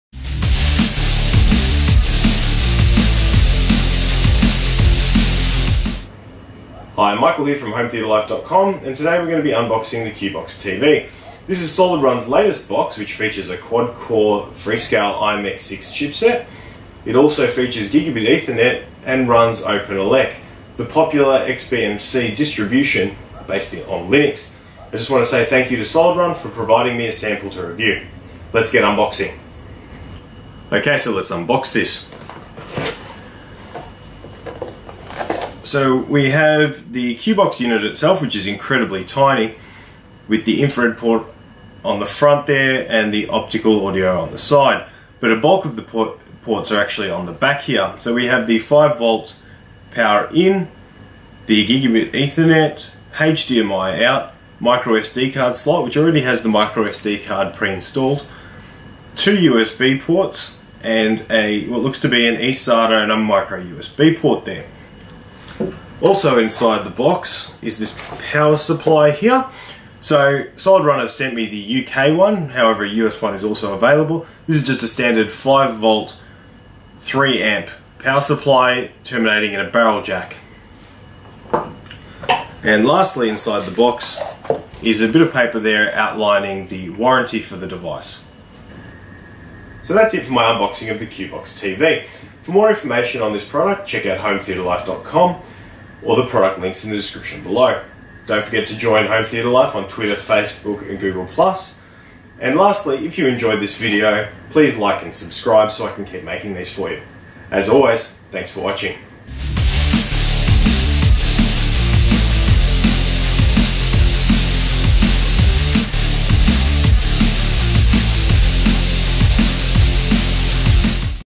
SolidRun CuboxTV Unboxing
The SolidRun CuboxTV is a quadcore Linux HTPC powered by a Freescale i.MX6 chipset. It's also armed with Gigabit Ethernet and built-in IR reciever.

Thank you to SolidRun for providing me a sample to review

---Technical Specs---
Chipset: Freescale i.MX6 with Vivante GC2000 3D GPU
RAM: 1 GB SDRAM
Storage: 8 GB + microSD
Video & Audio Output: HDMI 1.4, SP-DIF
Connectivity: Gigabit Ethernet
USB: 2 x USB 2.0 port
OS: Linux/OpenELEC

For more information and to get a SolidRun CuboxTV for yourself,